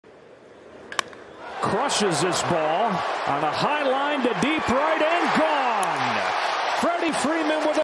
Only 2 Players Did This in 2022 | 2023 Fantasy Baseball
Fun Fact:

Only 2 hitters were among the top 10 in xwOBA against fastballs, breaking balls, and offspeed pitches - Min 50 PA:

Freddie Freeman
Yordan Alvarez